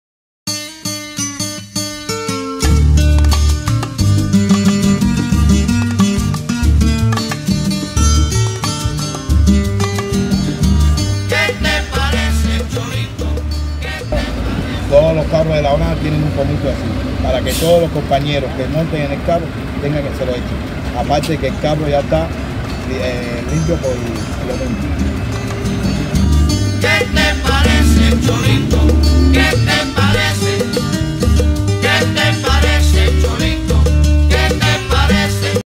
¿QUÉ TE PARECE EL HIPOCLORITO?
En la lucha contra la Pandemia del Coronavirus, un gran aliado es el HipocloritoDeSodio ¿Qué te parece?